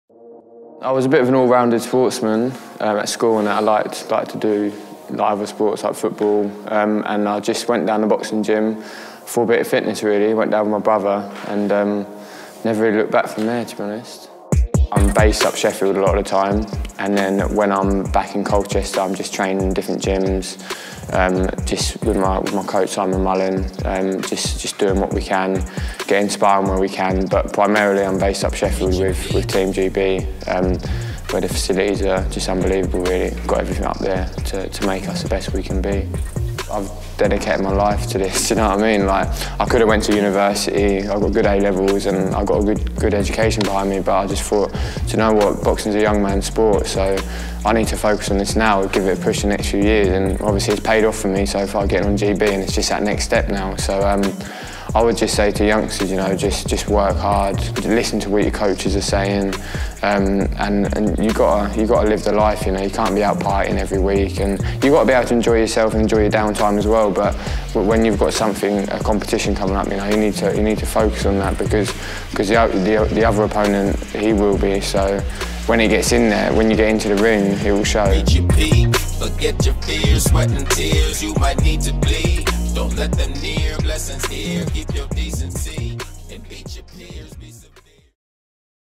Introduction To GB Boxer Lewis Richardson
We headed down to the gym to find out more about GB Boxer Lewis Richardson.